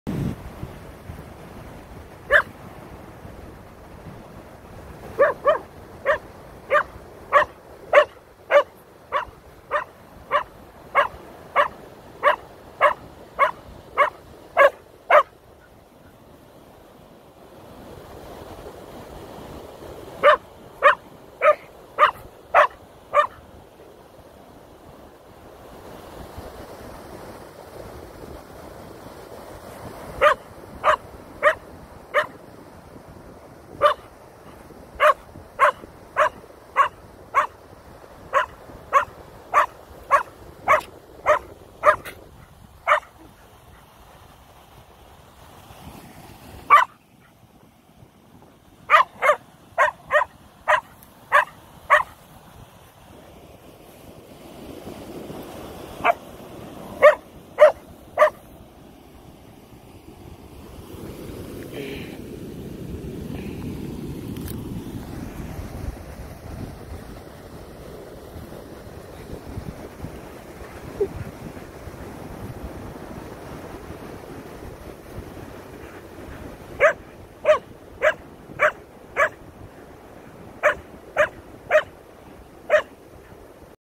Koda the Keeshond
Koda has a dislike/fear of feathers. Here is a hilarious video of his feather encounter.